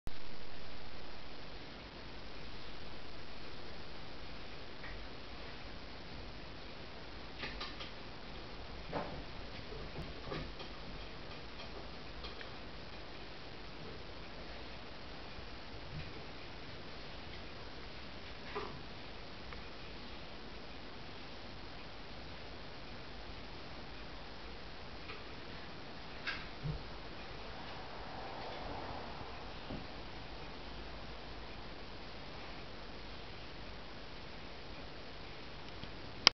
Rocking chair
Spense shows his disrespect for the chair